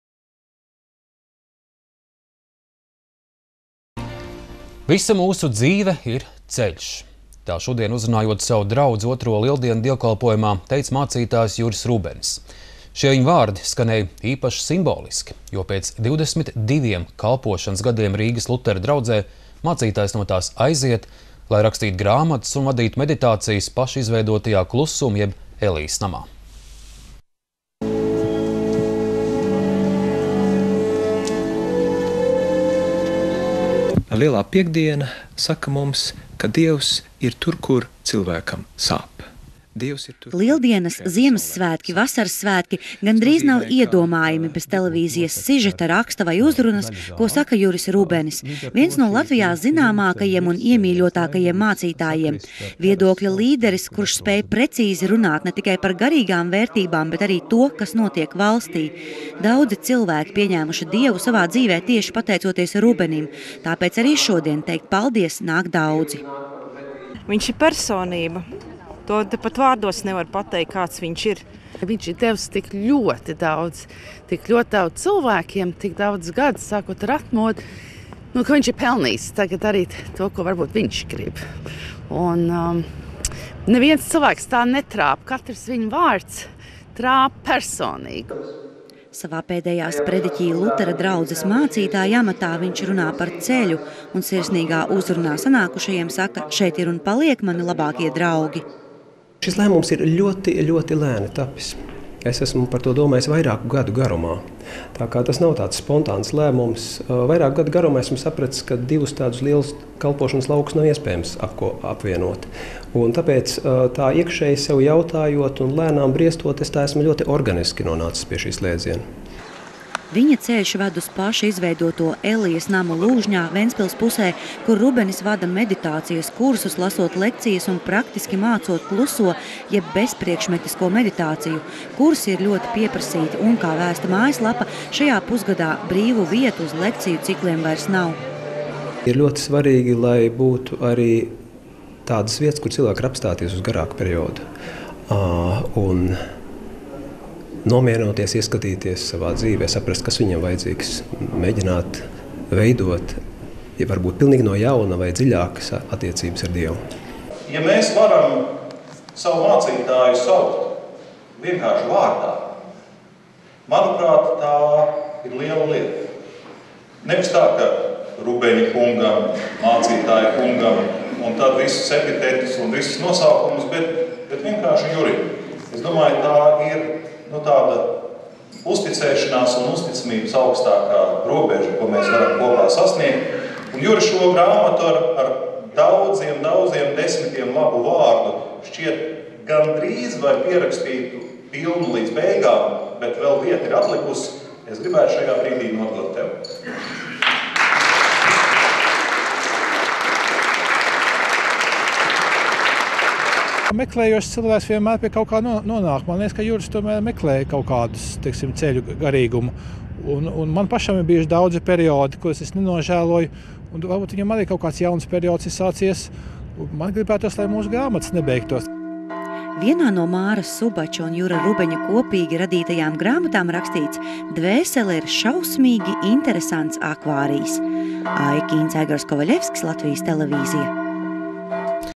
Juris Rubenis aiziet no Rīgas Lutera draudzes
Visa mūsu dzīve ir ceļš! Tā šodien, uzrunājot savu draudzi 2. Lieldienu dievkalpojumā, teica mācītājs Juris Rubenis. Šie viņa vārdi skanēja īpaši simboliski. Jo pēc 22 kalpošanas gadiem Rīgas Lutera draudzē mācītājs no tās aiziet, lai rakstītu grāmatas un vadītu meditācijas paša izveidotajā Klusuma jeb Elijas namā.